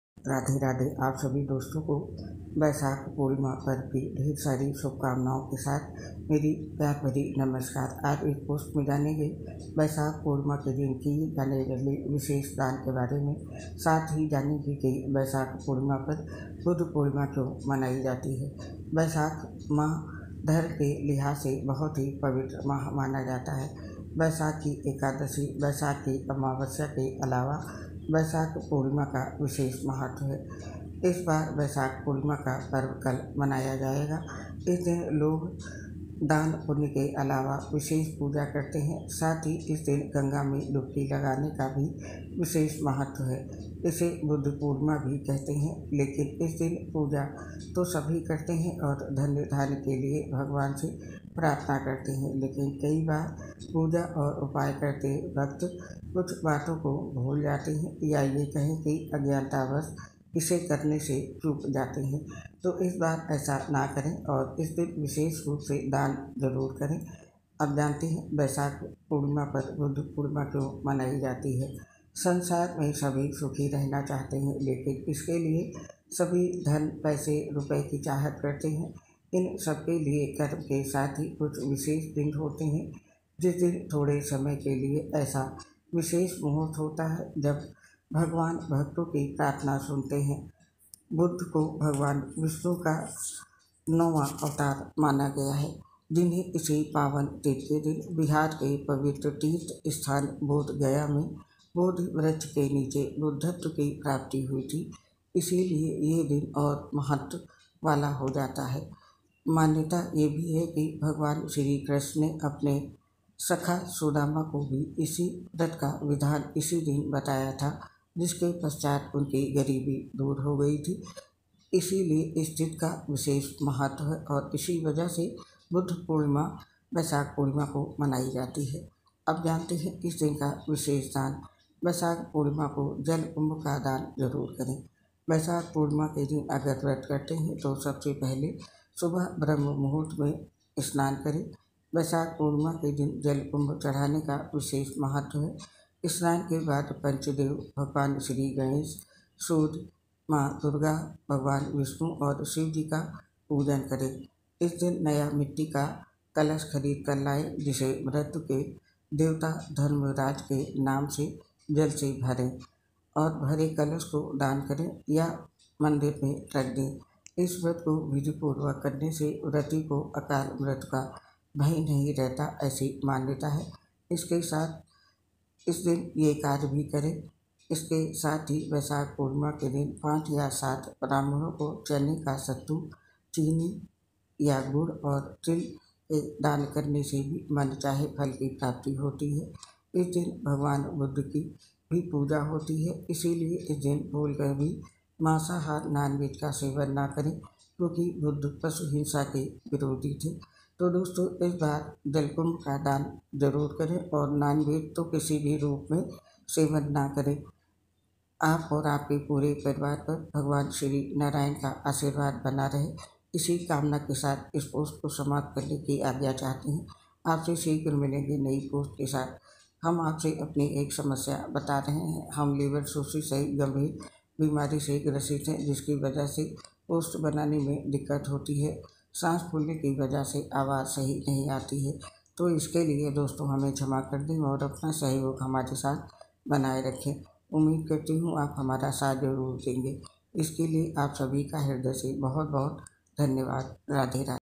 बैशाख पूर्णिमा 2024 के दिन. मिट्टी के घड़े मे इसे भरकर मंदिर या गरीब को दान डि.बाक्स में देखे डिटेल
ये जानकरी धार्मिक ज्योतिष शास्त्र पंचाग पर आधारित है
वीडियो तैयार किया@शैल फोटोग्राफ साभार सोशल साइट्स
बैशाख पूर्णिमा 2024 के दिन इसे मिट्टी के घड़े में भरकर मंदिर या गरीब को दान करें दूर होगी दरिद्रता और भरेगा धन का भंडार करे ये भी कार्य जानिए बैशाख पूणिॅमा को ही क्यो मनाई जाती है बुद्ध पूणिॅमा
पूजा पाठ करेंंट